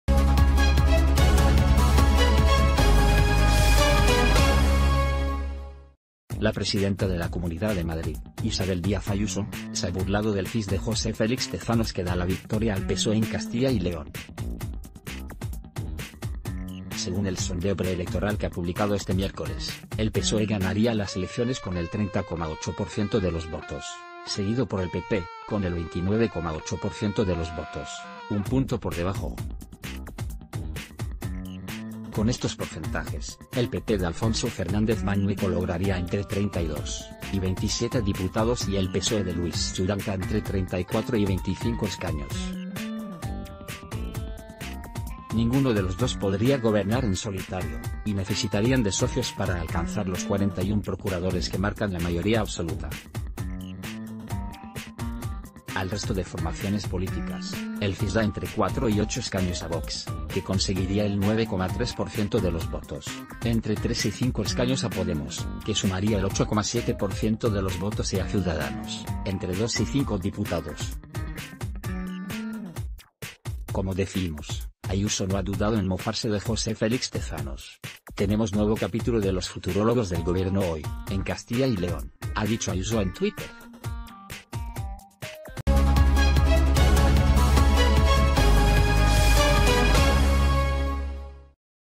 Ayuso se bur.la de Tezanos tras publicar un CIS que nadie cree: «Tenemos nuevo capítulo de los...
Ayuso se bur.la de Tezanos tras publicar un CIS que nadie cree: «Tenemos nuevo capítulo de los futurólogos»